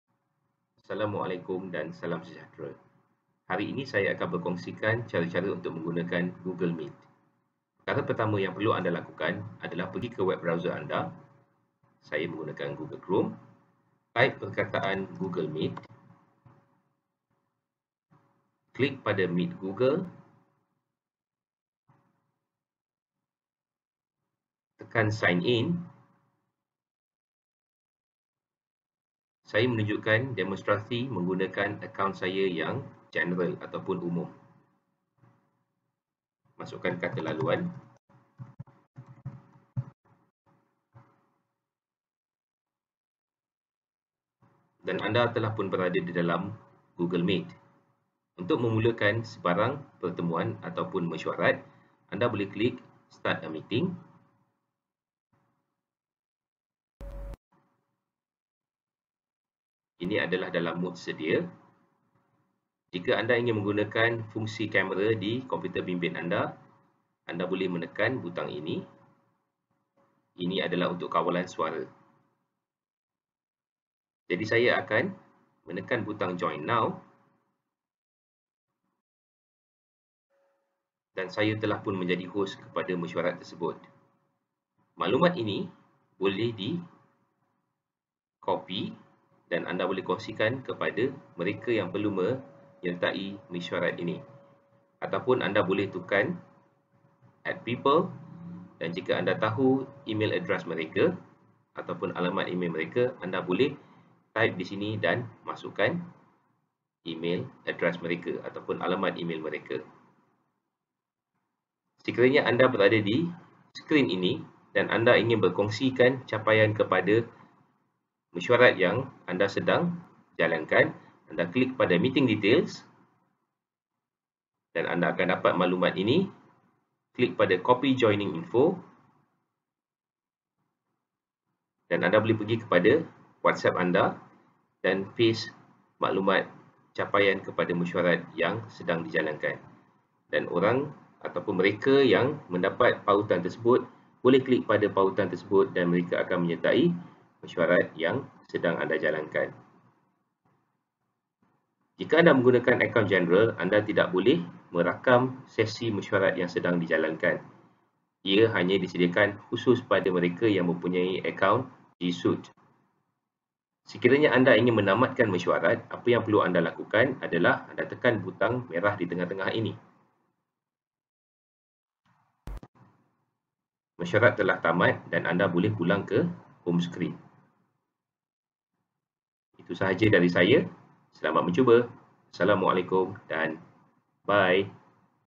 Google Meet (Akaun Google Biasa)
Cara menggunakan Google Meet bagi mereka yang menggunakan akaun Google Biasa